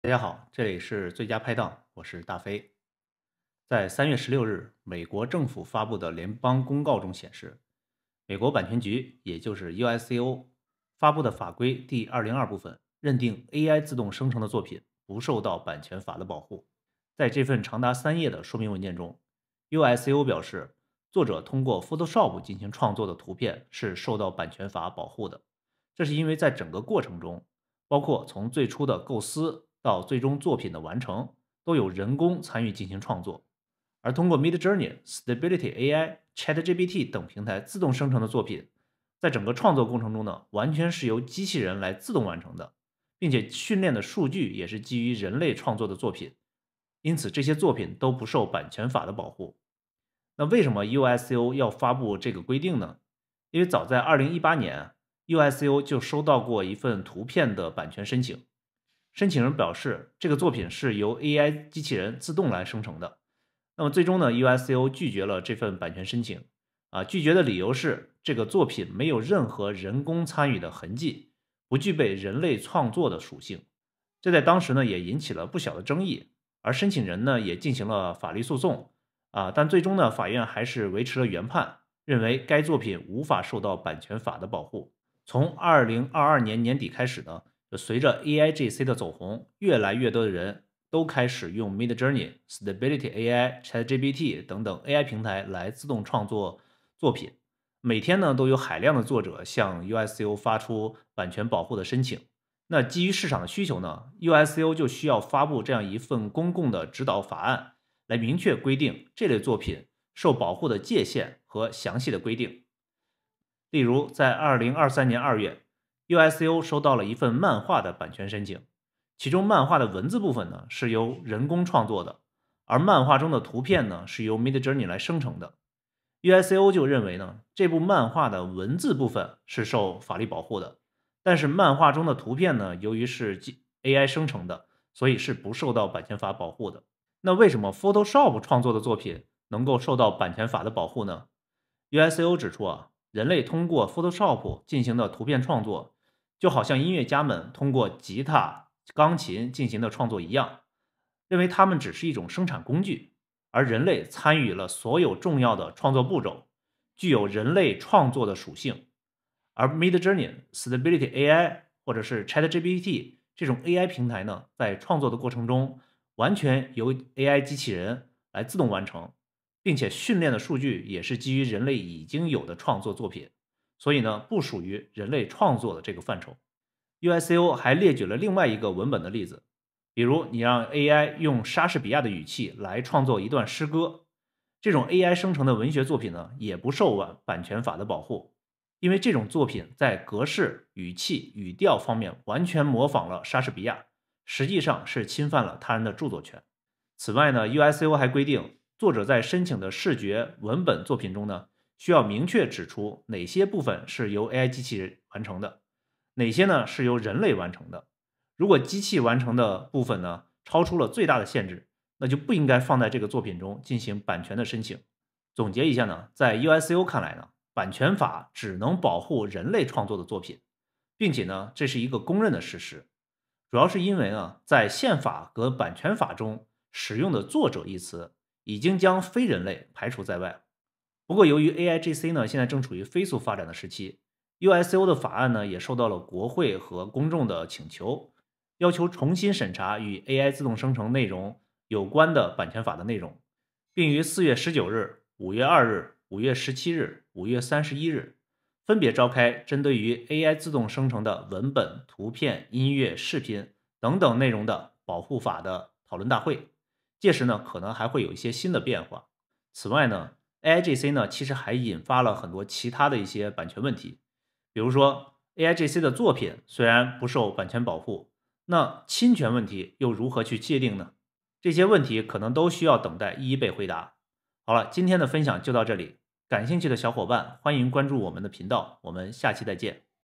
【AIGC】美国版权局最新规定：通过ChatGPT、Midjourney等AI自动生成的作品，不受版权法保护
在3月16日，美国政府发布的联邦公告中显示，美国版权局，也就是USCO，发布的法规第202部分，认定AI自动生成的作品，不受版权法保护。在这份长达三页的说明文件中，USCO表示，作者通过Photshop进行创作的图片作品是受保护的，这是因为在整个过程中，包括从最初的构思到最终作品的完成，有人工参与进行创作。而通过Midjourney、Stability AI、ChatGPT等平台自动生成的作品，在整个创作过程中完全由机器人自动完成，并且训练的数据是基于人类创作的作品，因此，不受版权法保护。